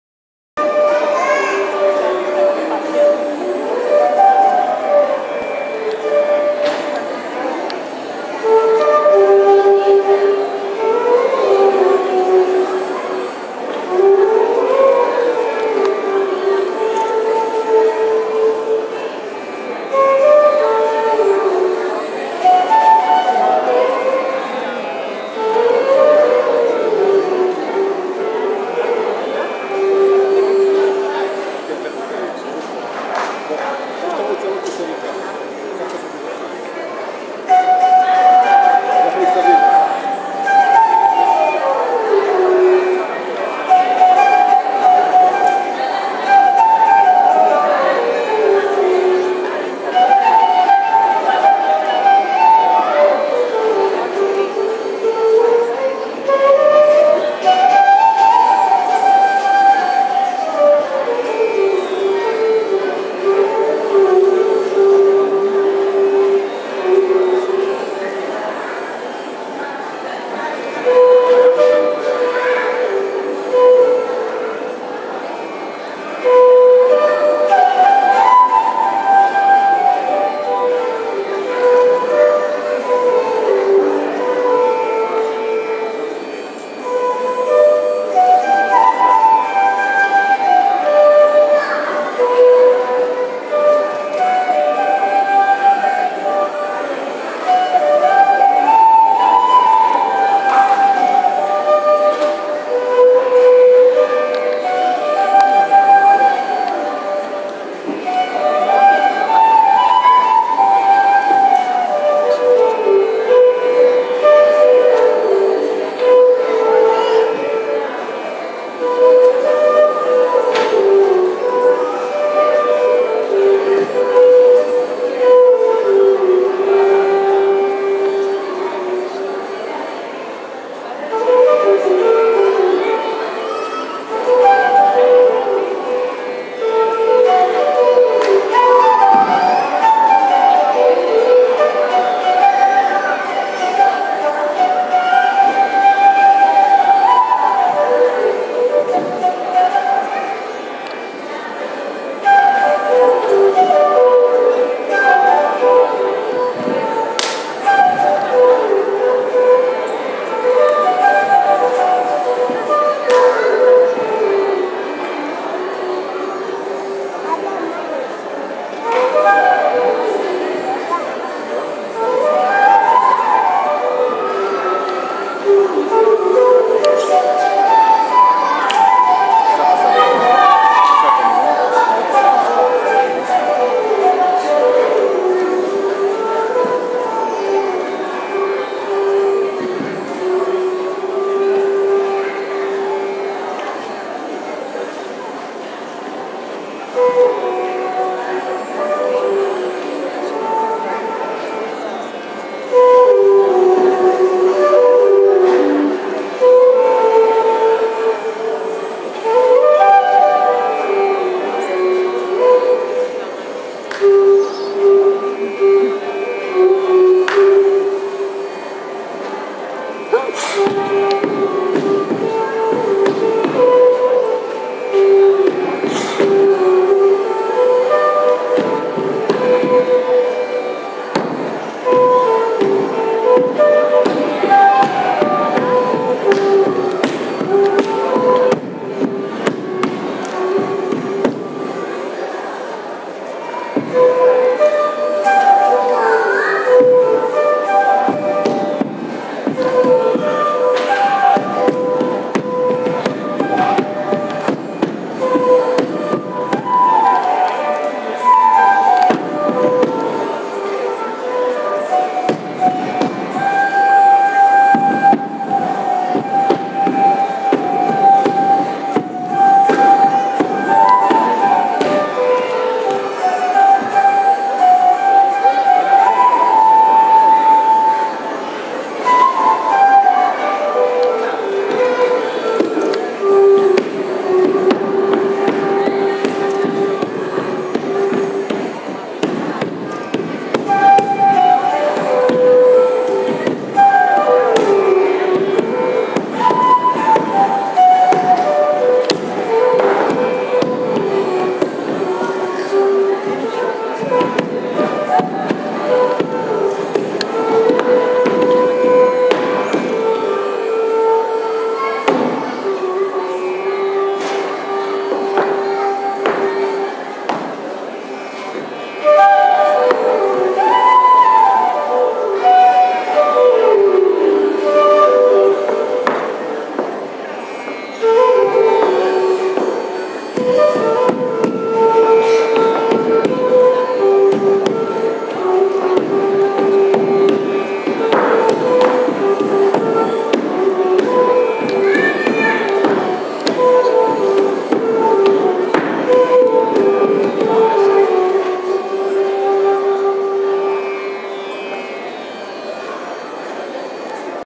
"Holy" - By Idan Cohen and Sapphire Creations
Thanks to Efrat Herman, meet Idan Cohen!

An Israeli choreographer, who's co-produced with Sapphire Creations - a dance company based in Kolkata - "Holy", an interactive dance piece created for A TASTE OF INDIA - a four days Indian festival in Jaffa's Machsan 2.

This is just a taste of "Holy".